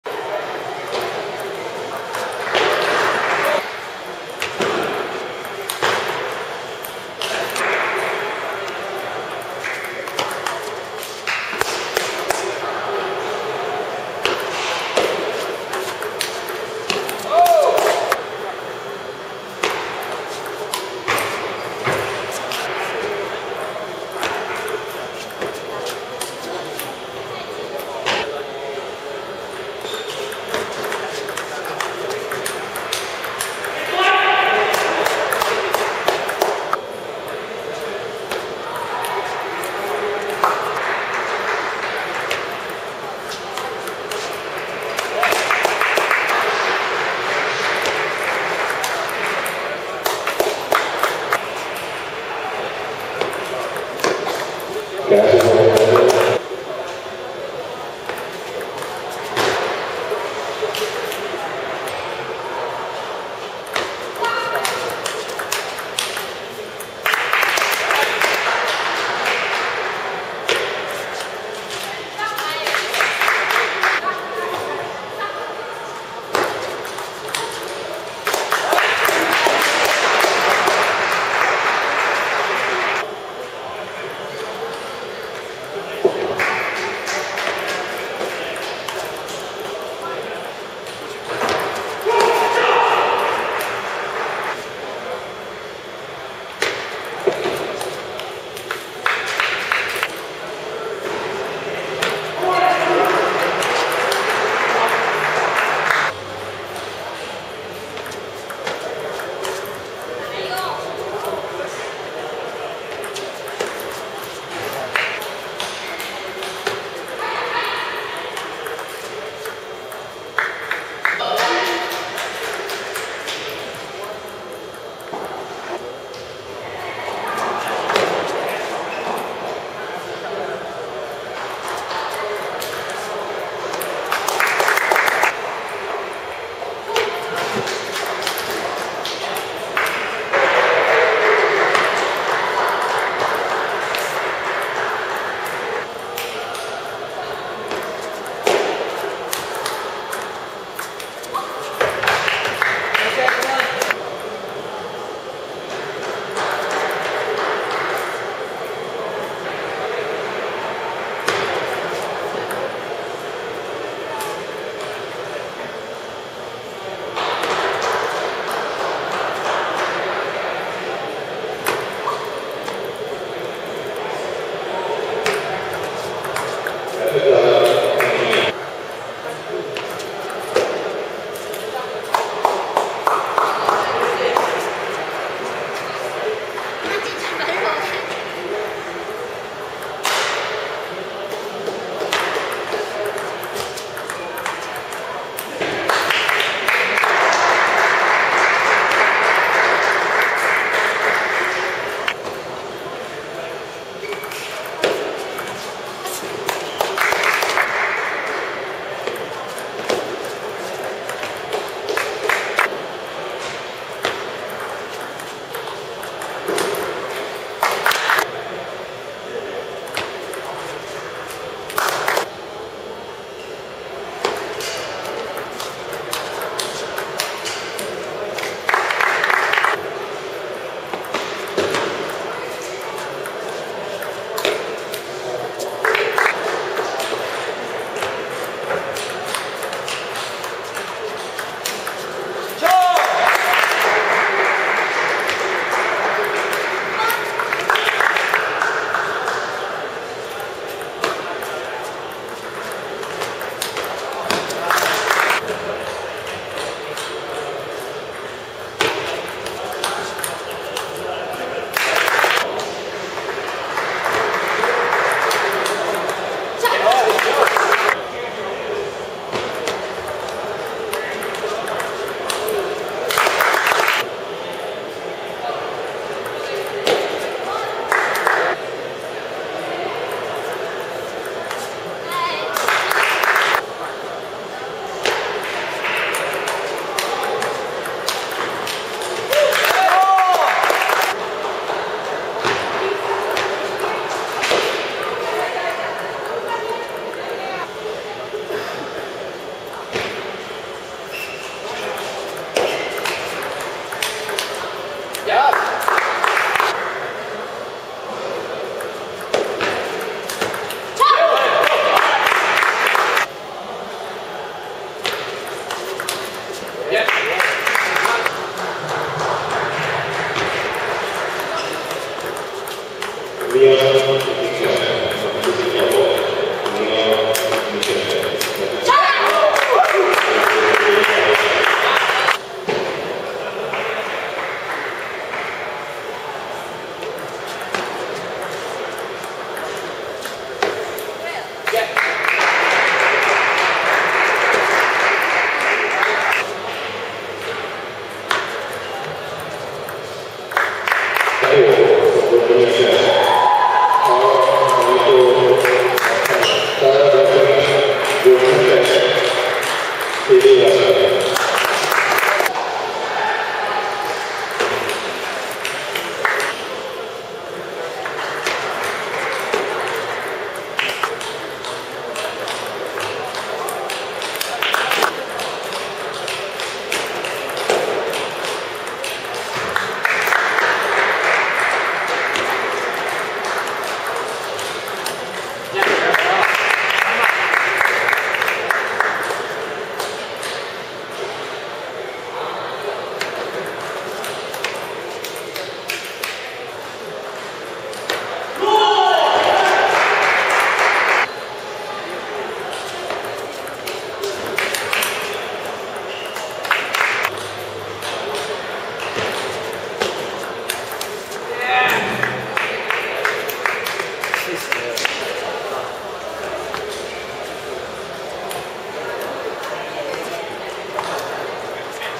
2013 NCTTA Nationals - Women's Singles (Round of 32)
2013 National Collegiate Table Tennis Association - National Championships
Rockford, Illinois

Quanying (Sally) Su, San Jose State
-vs-
Judy Chen, Texas